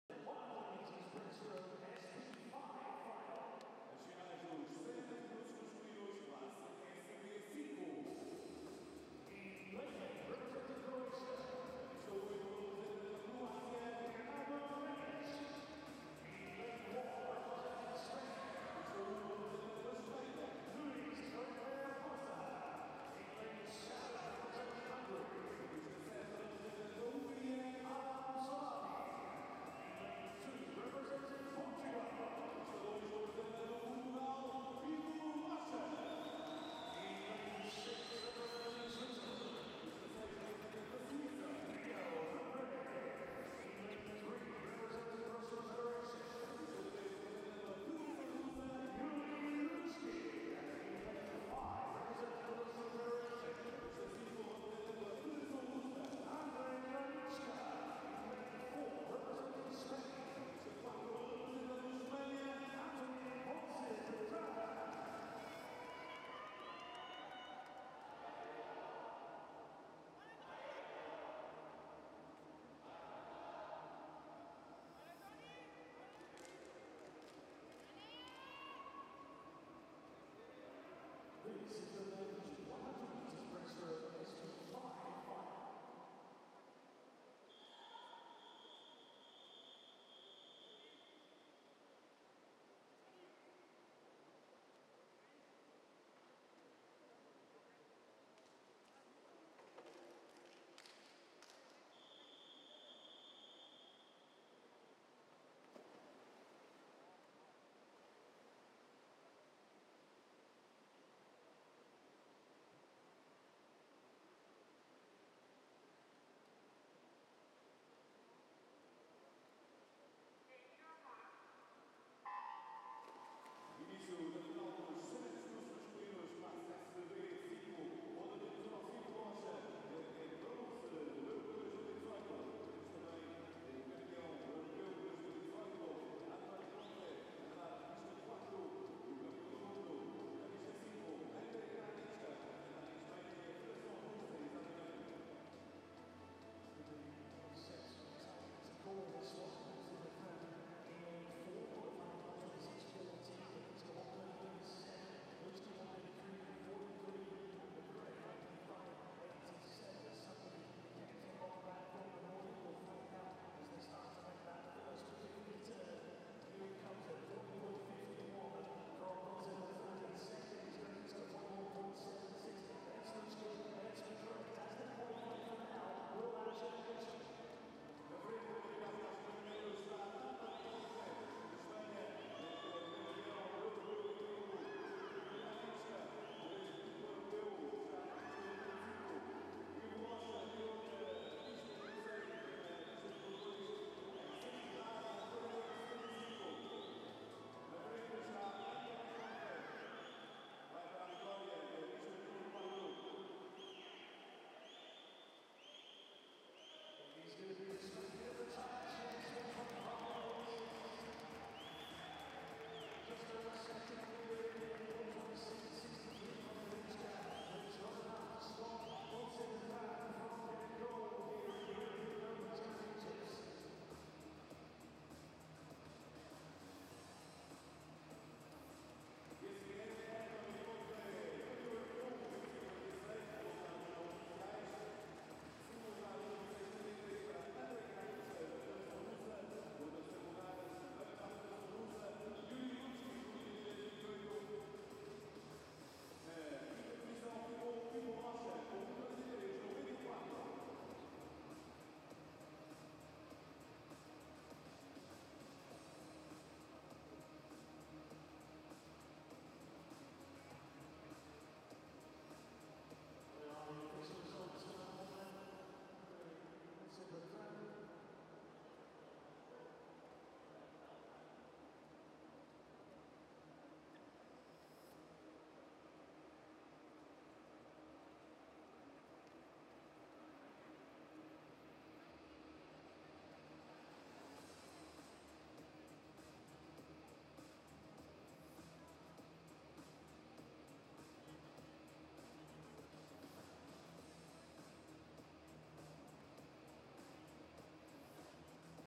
Men's 100m Breaststroke SB5 Final |World Para Swimming European Open Championships| Paralympic Games
Gold: Antoni Ponce Bertran
Silver: Andrei Granichka
Bronze: Iurii Kuchkin